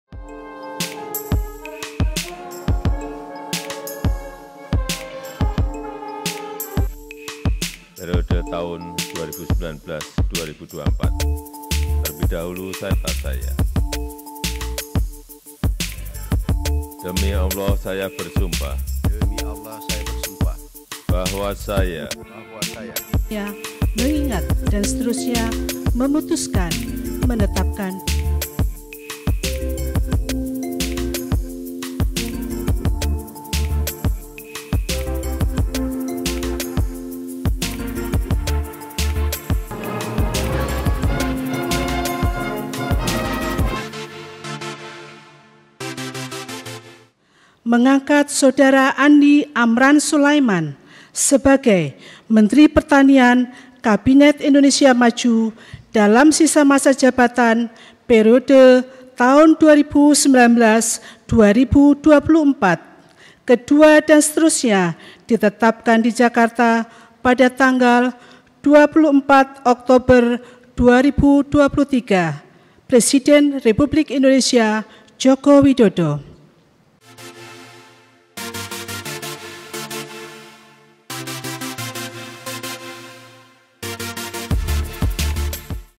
Amran Sulaiman Resmi Jadi Menteri Pertanian Gantikan SYL
Presiden Joko Widodo melantik  Andi Amran Sulaiman sebagai Menteri Pertanian di Istana negara jakarta pada Rabu pagi.

Amran dilantik menjadi Menteri Pertanian menggantikan Syahrul Yasin Limpo yang telah mengundurkan diri dari Kabinet Indonesia Maju.

Amran dilantik Jokowi berdasarkan Keppres nomor 101/P Tahun 2023. Sebelumnya Amran pernah menjadi Menteri Pertanian di kabinet pemerintahan Jokowi periode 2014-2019.